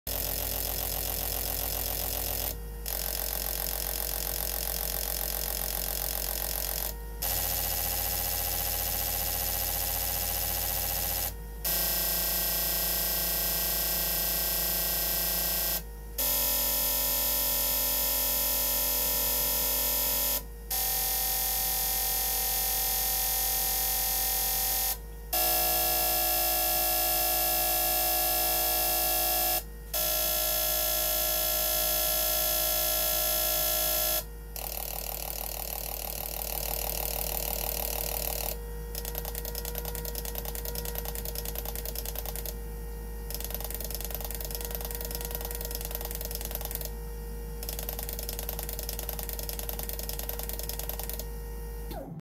2012 Dodge Grand Caravan 3.6 V6 05184185AC injectors spray pattern test at Toronto Injectors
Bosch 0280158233